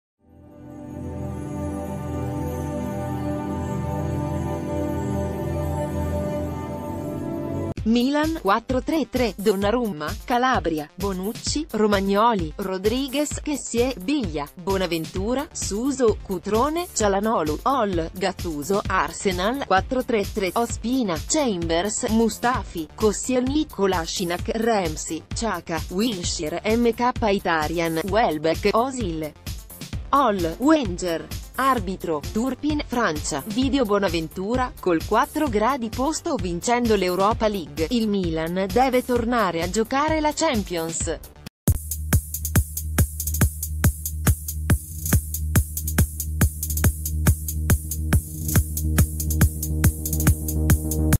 Ultime notizie | Le formazioni ufficiali di Milan-Arsenal: Gattuso sceglie i titolarissimi, Wenge...
Ultime notizie | Le formazioni ufficiali di Milan-Arsenal: Gattuso sceglie i titolarissimi, Wenger punta su Welbeck

Milan (4-3-3): Donnarumma; Calabria, Bonucci, Romagnoli, Rodriguez; Kessié, Biglia, Bonaventura; Suso, Cutrone, Calhanoglu. All.: Gattuso. Arsenal (4-3-3): Ospina; Chambers, Mustafi, Koscielny, Kolasinac; Ramsey, Xhaka, Wilshere; Mkhitaryan, Welbeck, Ozil. All.: Wenger. Arbitro: Turpin (Francia) Video - Bonaventura: "Col 4° posto o vincendo l'Europa League, il Milan deve tornare a giocare la Champions"